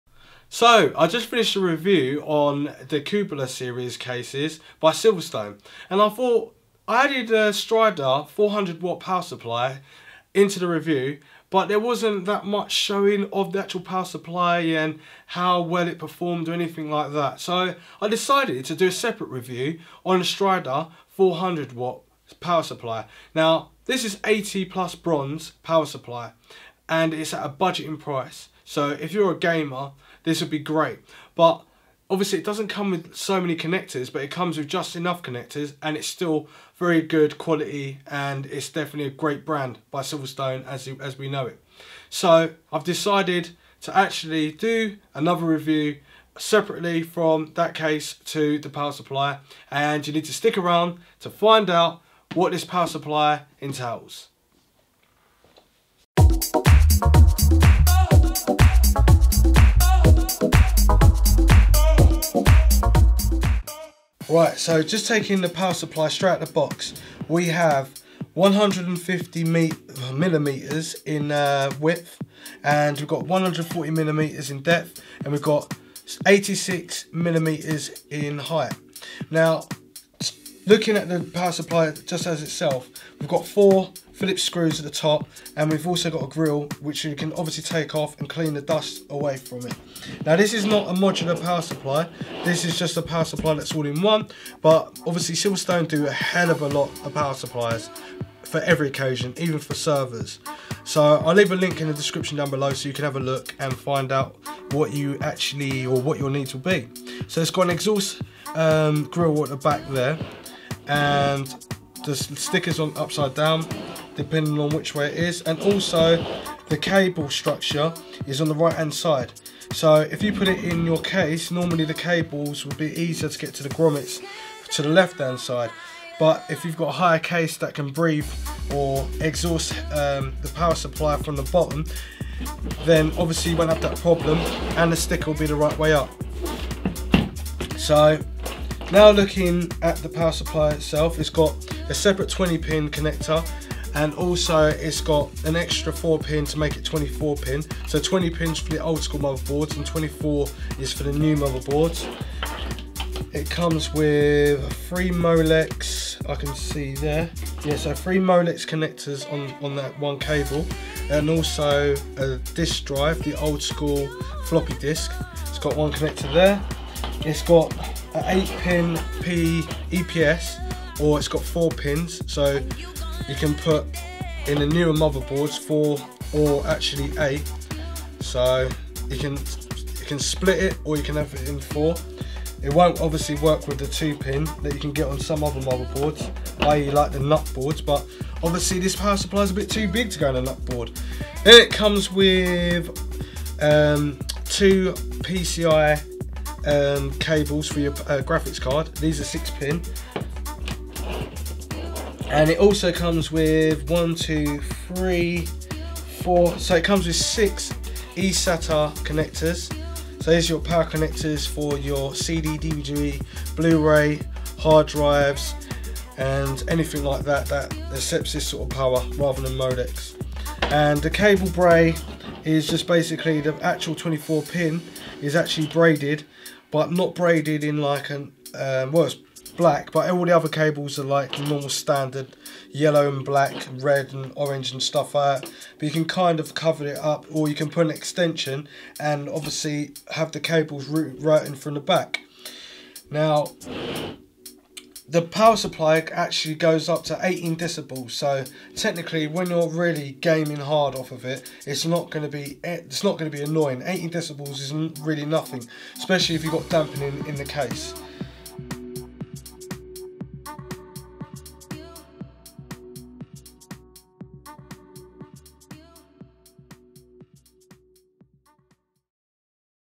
SilverStone Strider Essential Series 80 Plus 400W!? Review
TECH SPECS:
⭐️ High efficiency with 80 PLUS certification
⭐️ 400W 24hour continuous power output with 40℃operating temperature
⭐️ Class-leading single +12V rail with 25A
⭐️ Multiple protection circuitry
⭐️ Silent running 120mm fan with 18dBA minimum
⭐️ Supports ATX 12V 2.3
⭐️ Active PFC